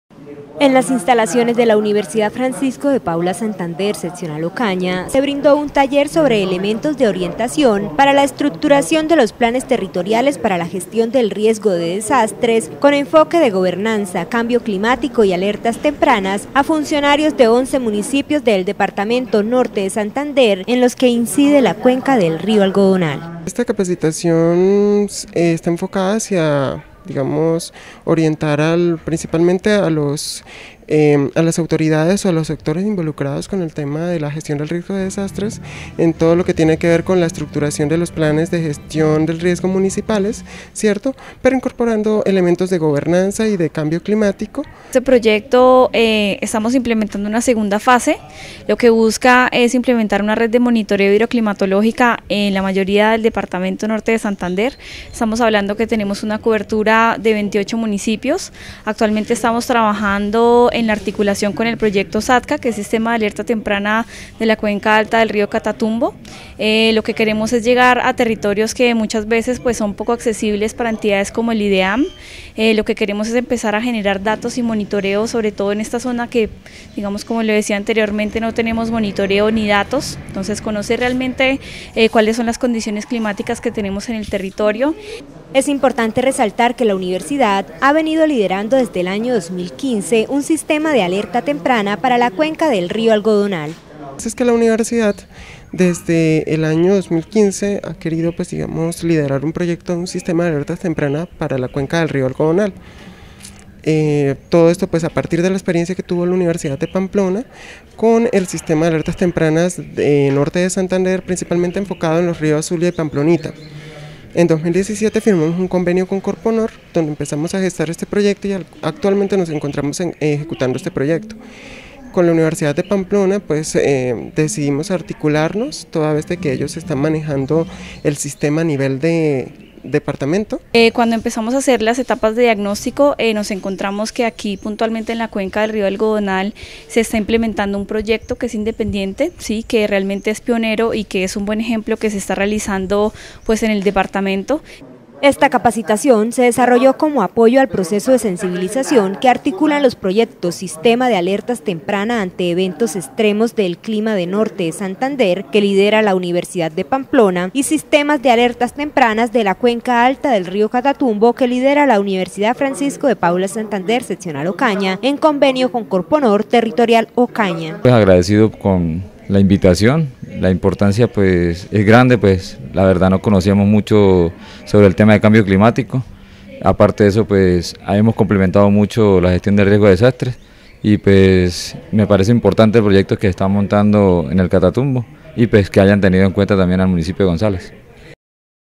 Nota Informativa - Capacitación cambio climático
Capacitación sistema de alerta temprana y adaptación del cambio climático.